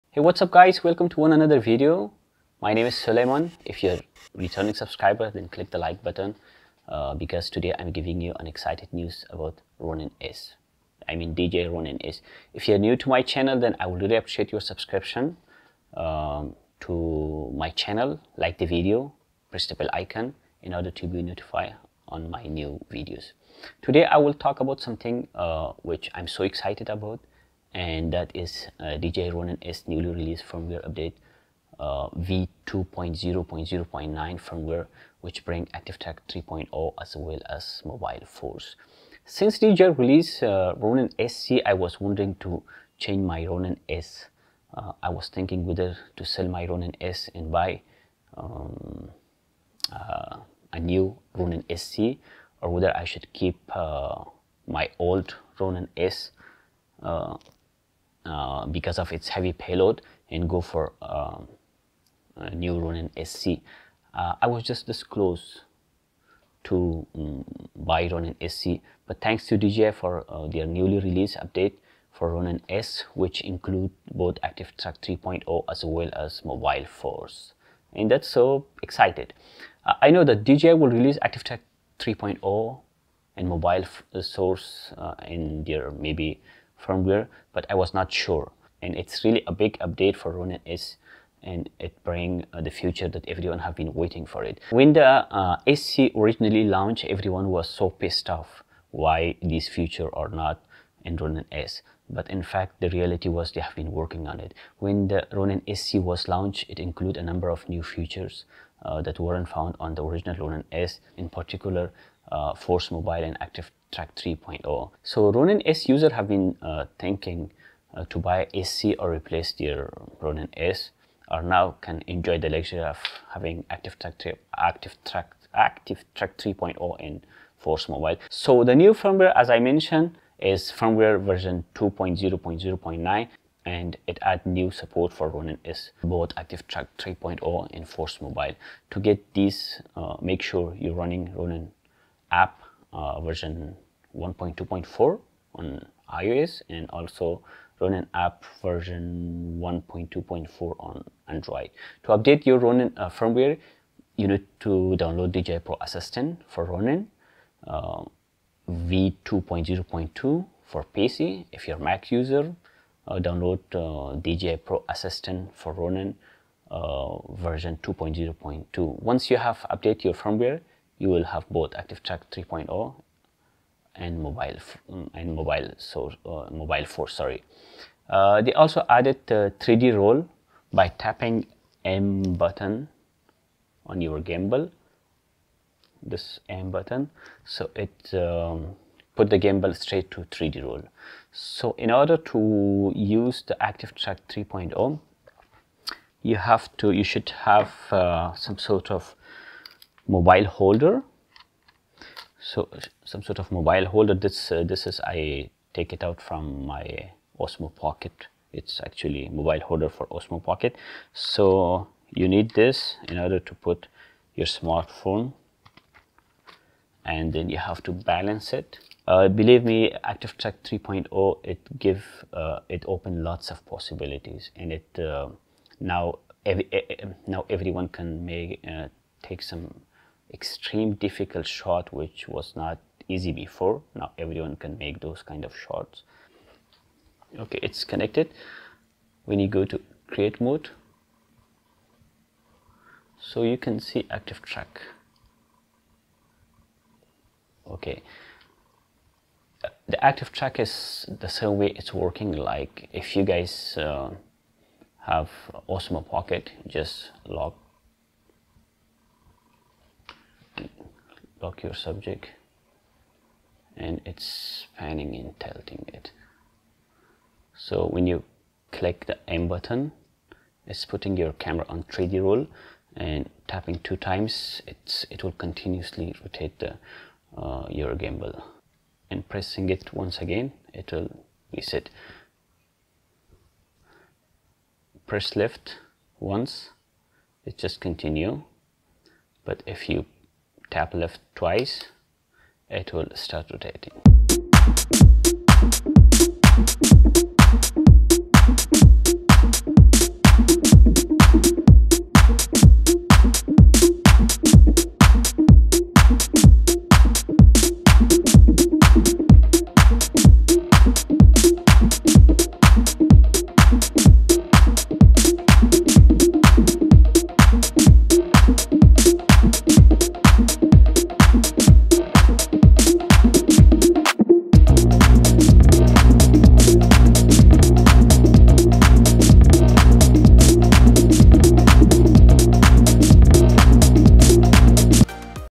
Ronin S - Active Track 3.0 & Force Mobile - Firmware V2.0.0.90
DJI newly released  Firmware Update for the Ronin-S that enables you to have Active Track 3.0 and Force Mobile and also 3d Roll on your ronin-s.